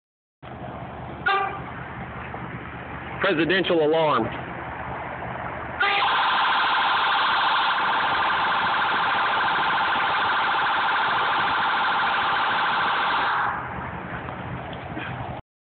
501 Tire & Wheel N. Little Rock, Arkansas "Loud Car Alarm"
the loudest car alarm i have ever heard! shake the building loud!